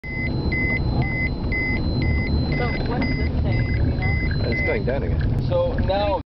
Chernobyl - Crossing a Bad Zone - Geiger Counter Goes Bonkers
Driving through a highly radioactive area in Chernobyl exclusion zone on the way to the town of Pripyat.  geiger Counter goes bonkers.  We are all getting nuked at the moment!